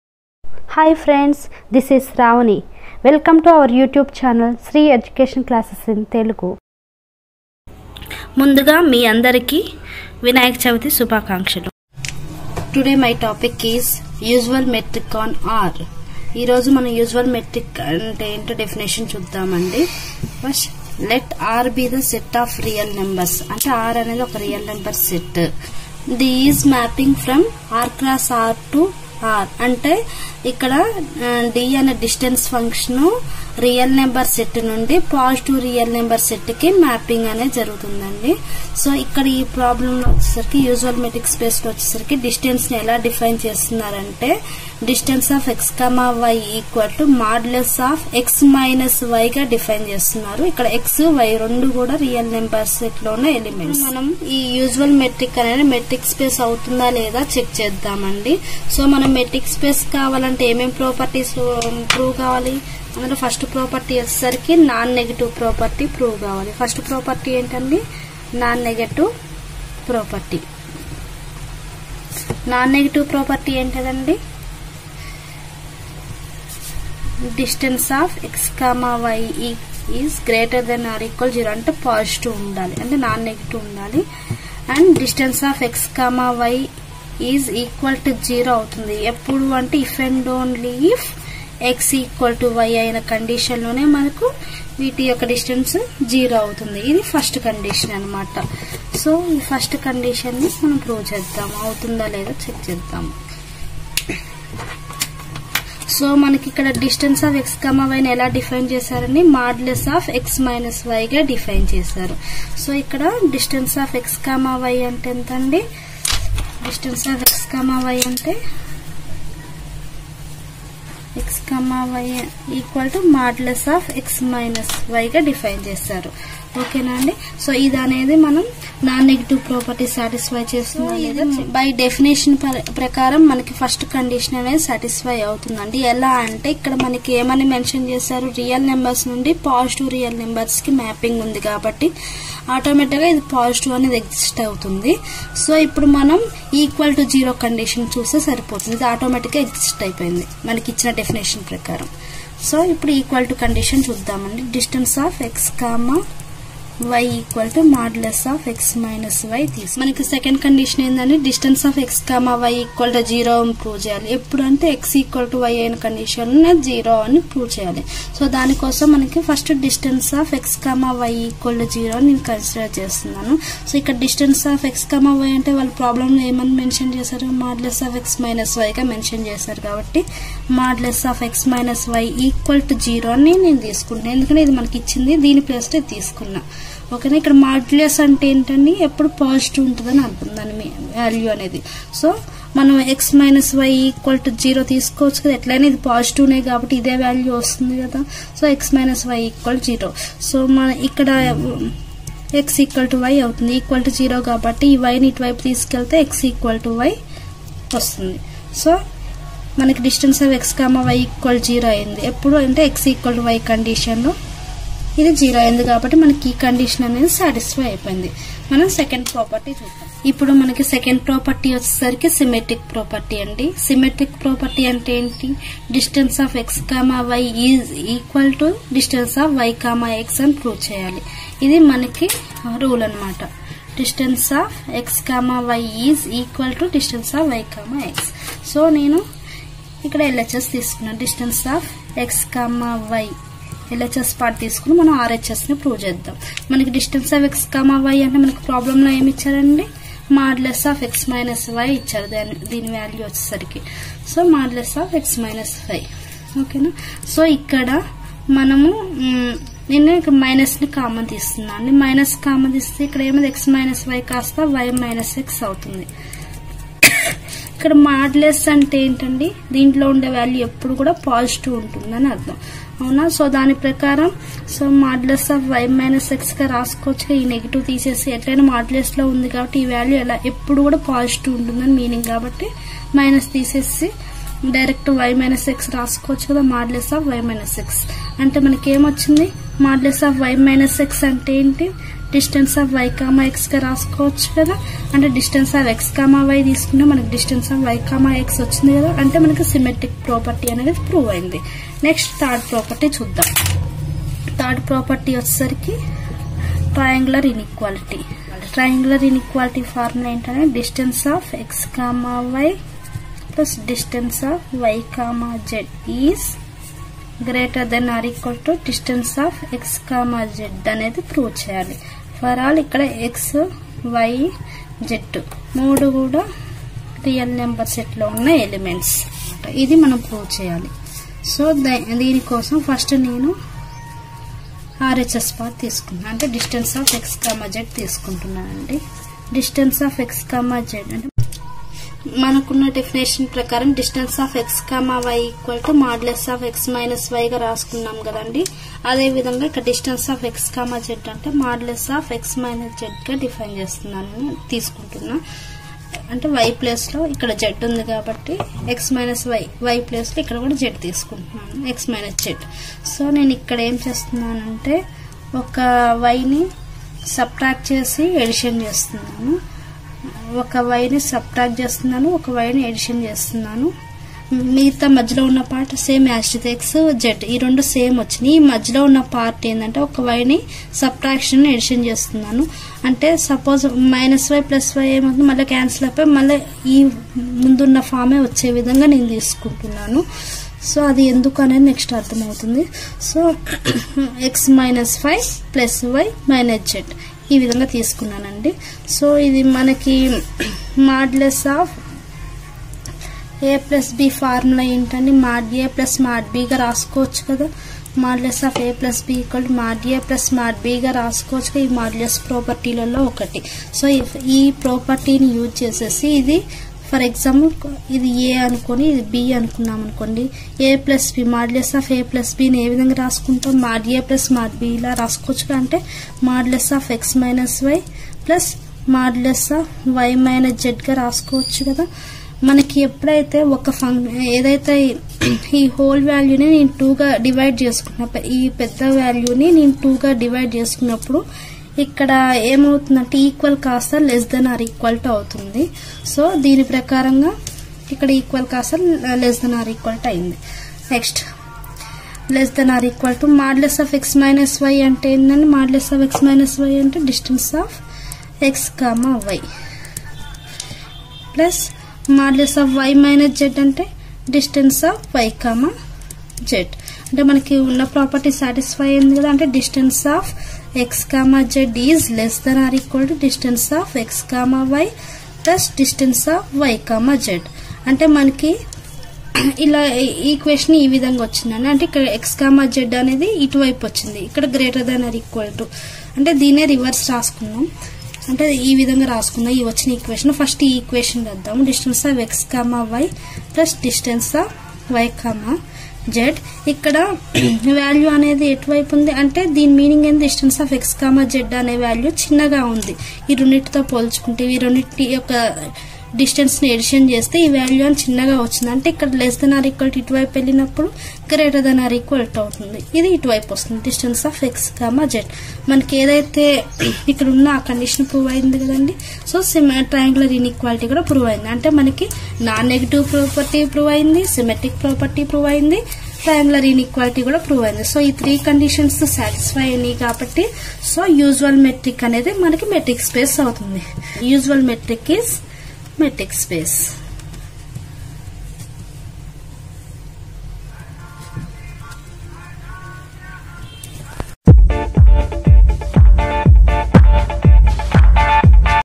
Usual metric on R defination || Metric space in telugu
HI FRIENDS ,
     WELCOME TO SRI EDUCATION CLASSES IN TELUGU,
     In this video i explained Usual metric on R defination,example of metric space